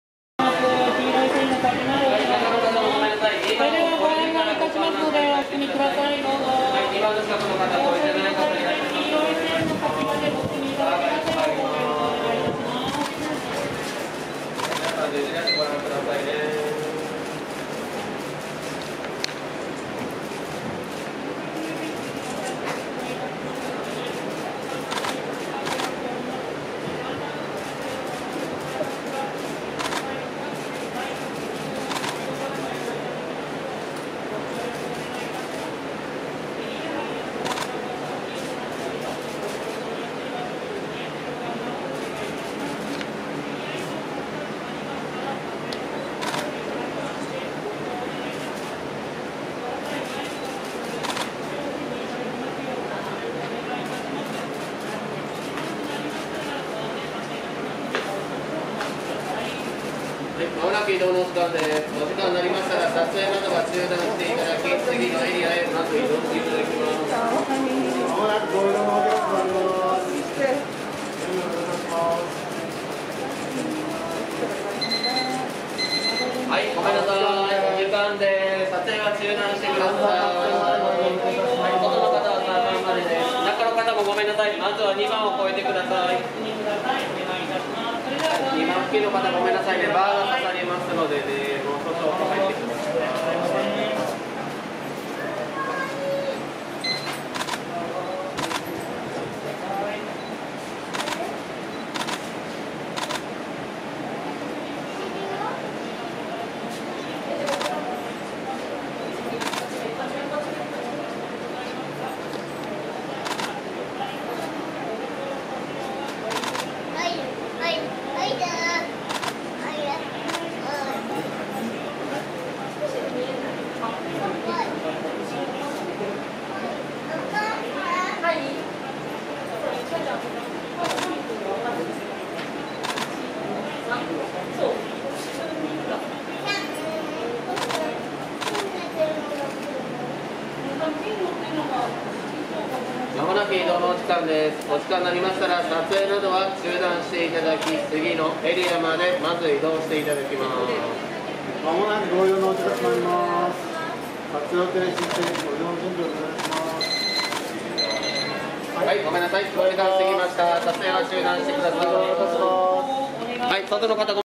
2022.9.22 THU 上野動物園 ジャイアントパンダ 双子パンダ シャオシャオ＆レイレイ 母シンシン 二刀流カメラ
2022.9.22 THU

上野動物園　ジャイアントパンダ　双子パンダ　シャオシャオ＆レイレイ　母シンシン

二刀流カメラ

レイレイは木で遊んでおりました。

シャオシャオは木でグッタリとして寝ておりました。

シンシンはうつぶせで寝ておりました。